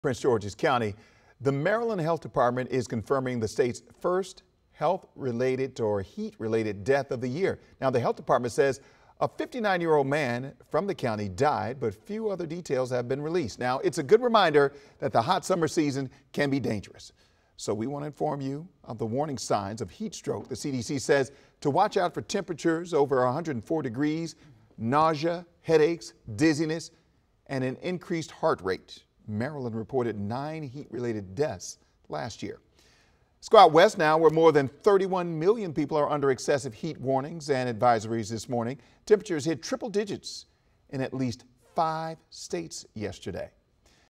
First heat-related death of the year reported in Maryland
The health department says a 59-year-old man has died.